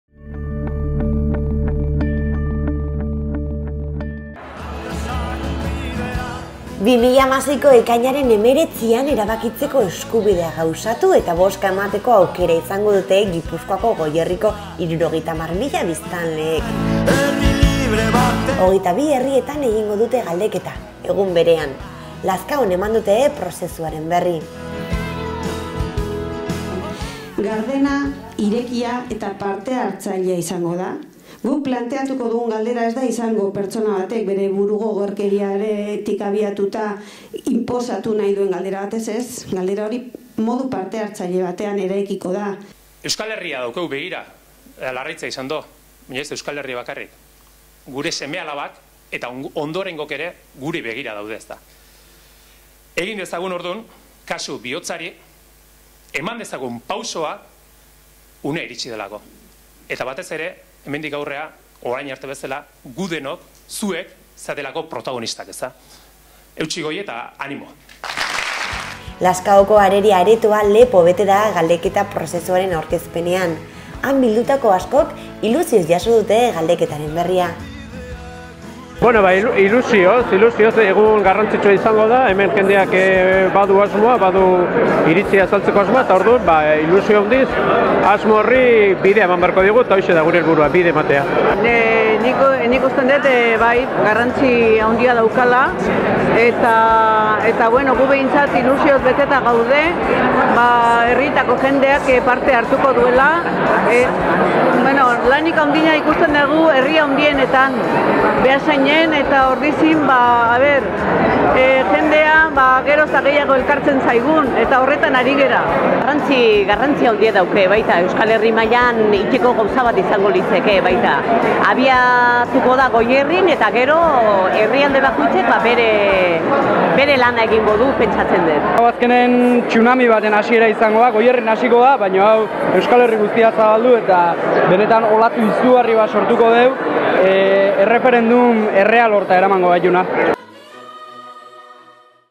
BERRIA. Herri galdeketaren aurkezpena Goierrin
Ekainaren 19an izango dira herri galdeketak Gipuzkoako 22 herritan. Lazkaon egin dute aurkezpen ekitaldia.